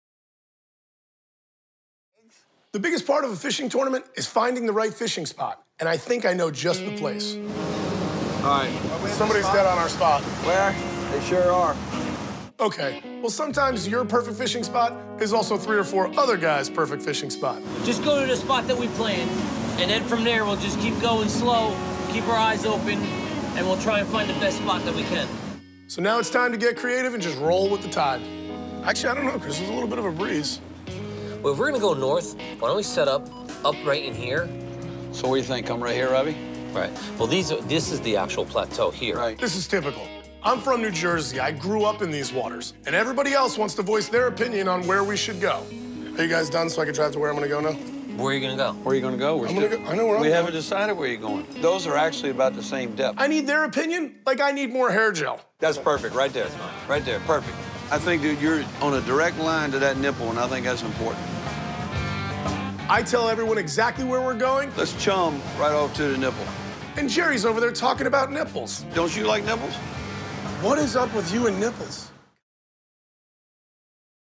Fish Mavericks Jerry Picks a fishing spot with Nipples
Jerry can't stop thinking about nipples